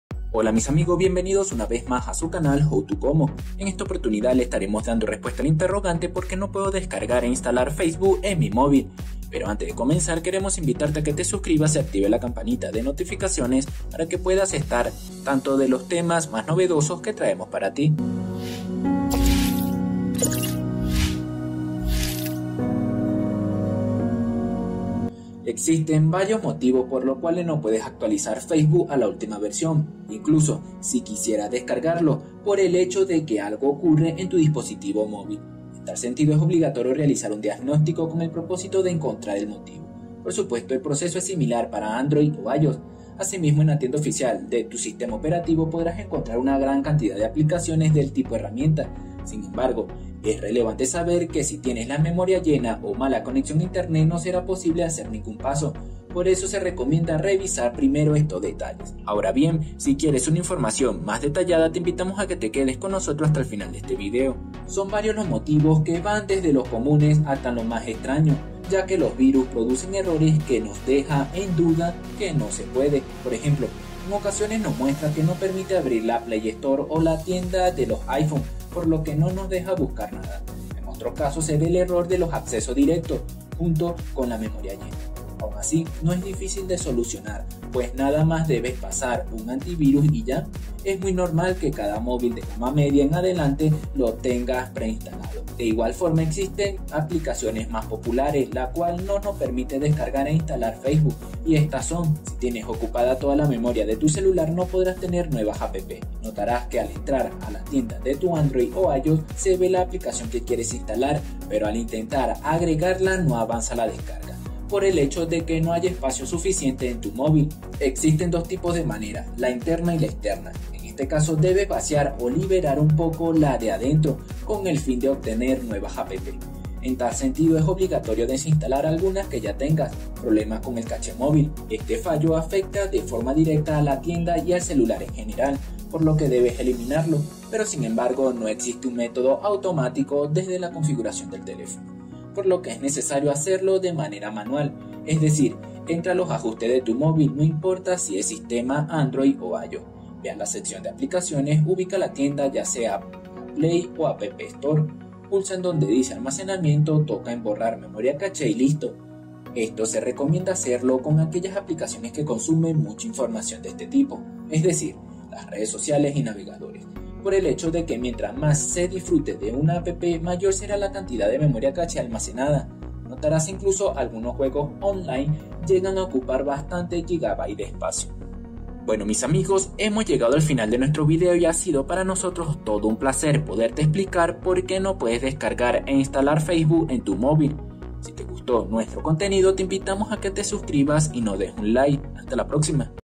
¿Por qué no puedo descargar e Instalar FACEBOOK en mi móvil? - Solución para Android e iOS
En este video te mostramos Por qué no puedo descargar e Instalar FACEBOOK en tu móvil. En este canal encontrarás los mejores tutoriales. ¡No te lo pierdas!